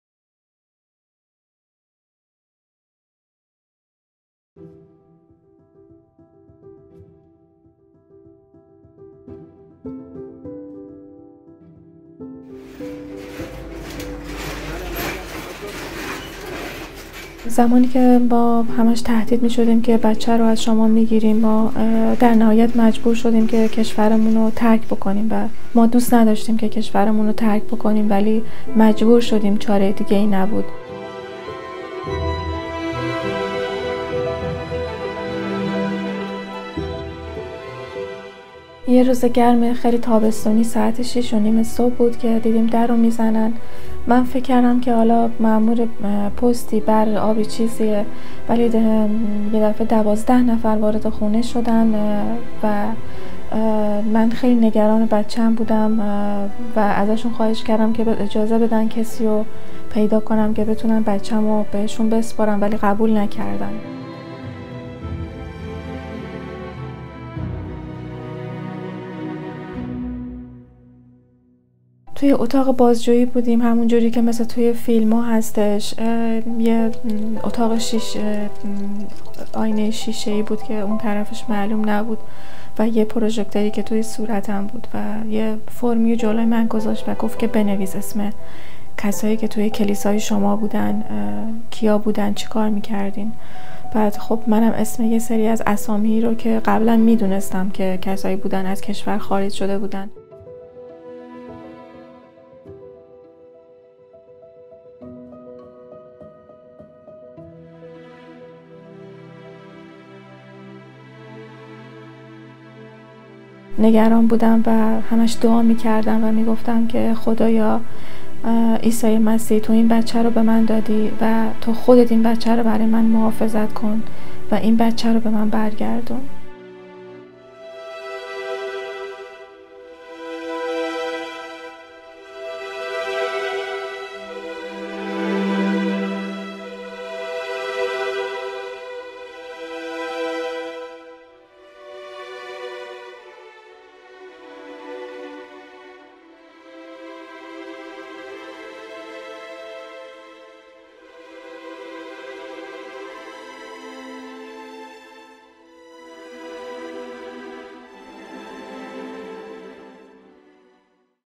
Vulnerable Young Women | Simin's Story
When Simin decided to follow Jesus, it gave her life meaning and hope, but also brought danger.